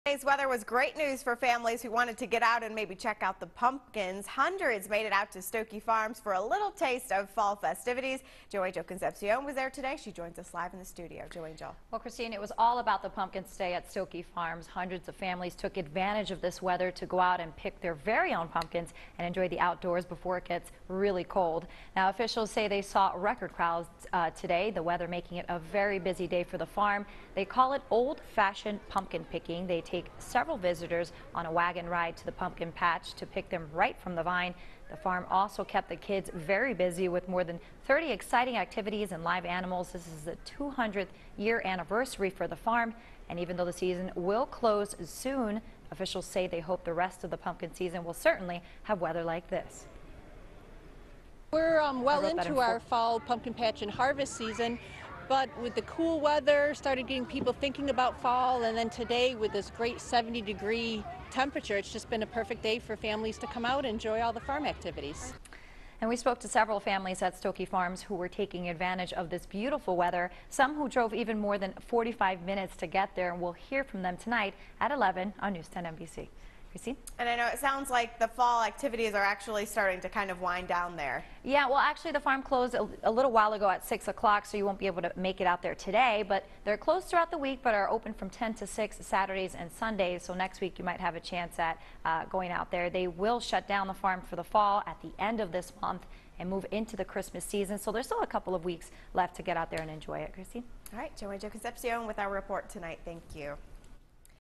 Hundreds pick pumpkins the "old fashioned" way at Stokoe
Sunday's weather was great news for families who wanted to check out the pumpkins. Hundreds made it out to Stokoe Farms for a little taste of some fall festivities.It was all about the pumpkins on Sunday at Stokoe Farms. Families took advantage of the weather to go out and pick their very own